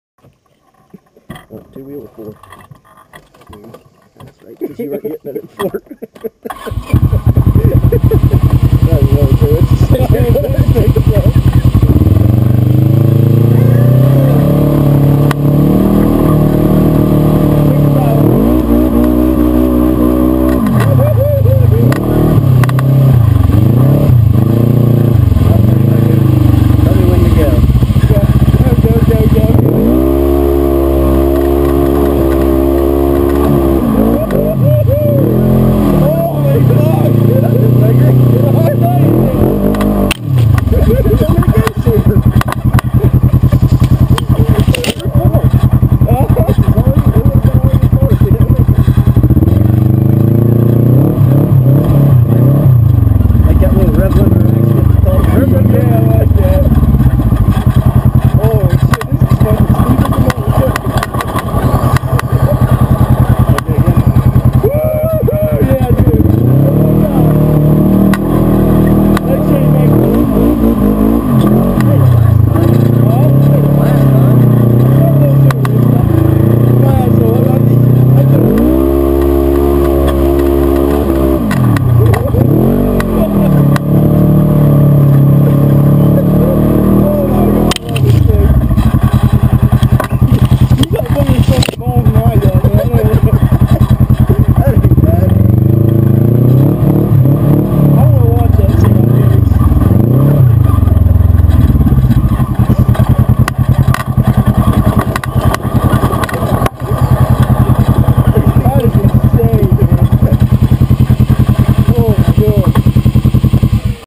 Polaris RZR 900XP HILL CLIMB
As the title says a few short climbs with a buddy of mine.